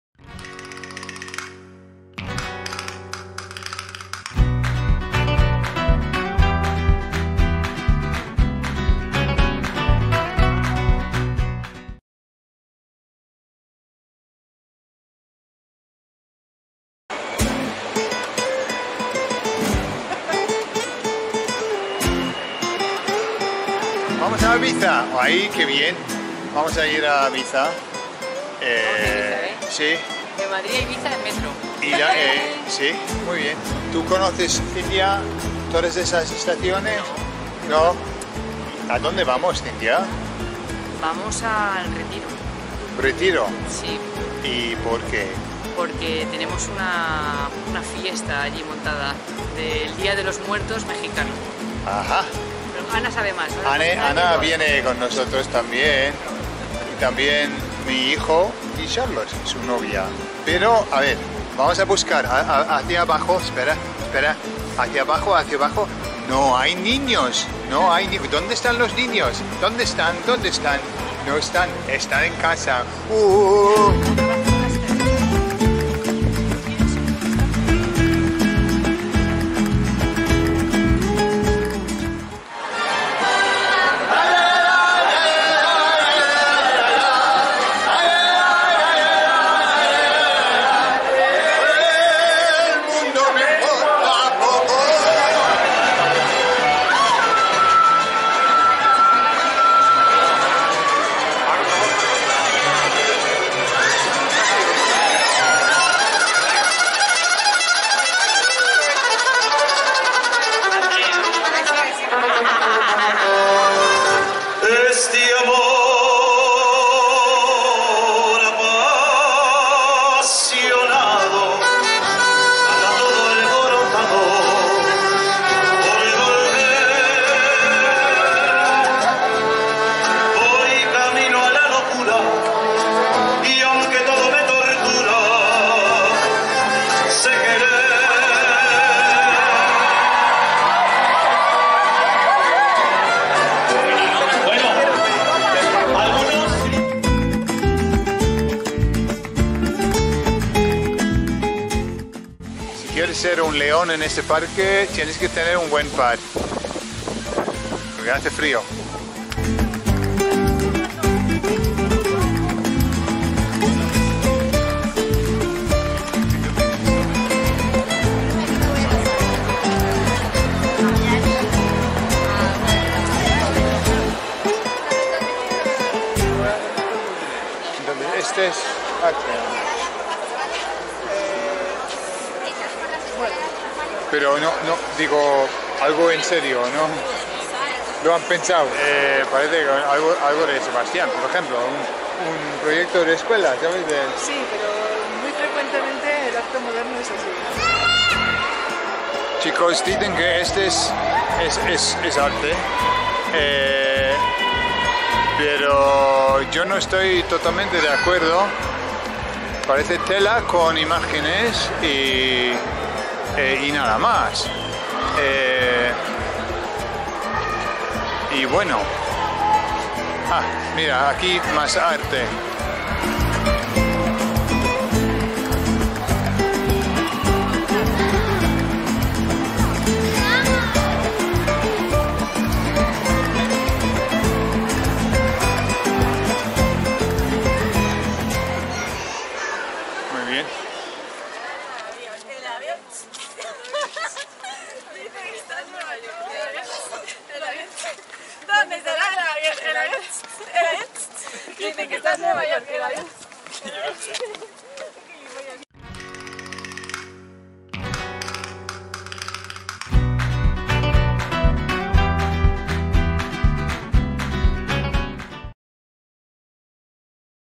Gordon's Diaries El día de los muertos LightSpeed Spanish
we are pleased to offer you a look into our special day of the dead celebration in Madrid.